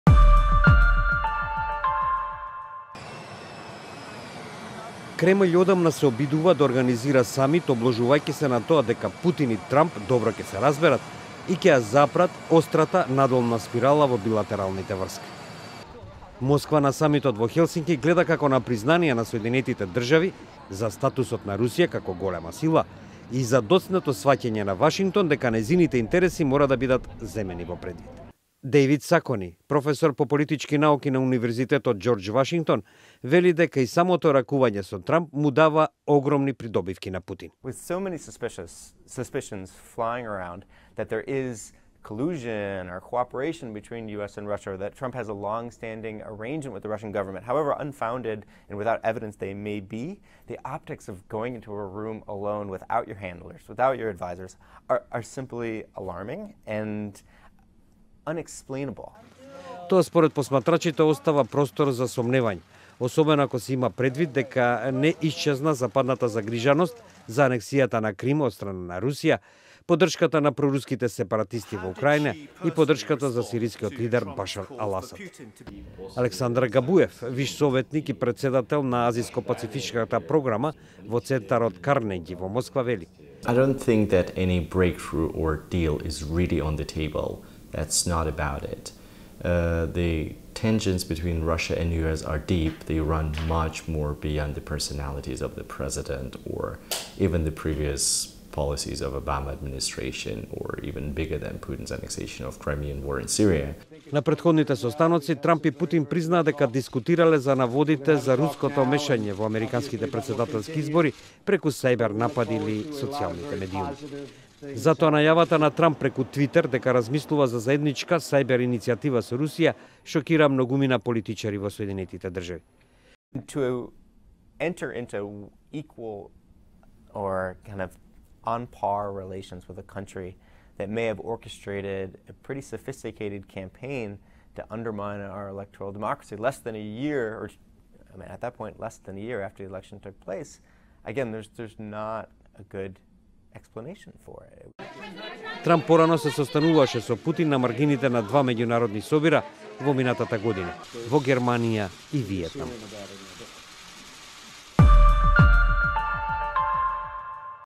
Самит кој може да значи многу и за Путин и Трамп
За американскиот претседател Доналд Трамп, средбата со Владимир Путин во Хелсинки претставува своевидна опасност со оглед на политичките реакции дома и во странство, велат аналитичарите. Според нив, за рускиот претседател, фактот што самитот се случува е веќе голема геополитичка победа.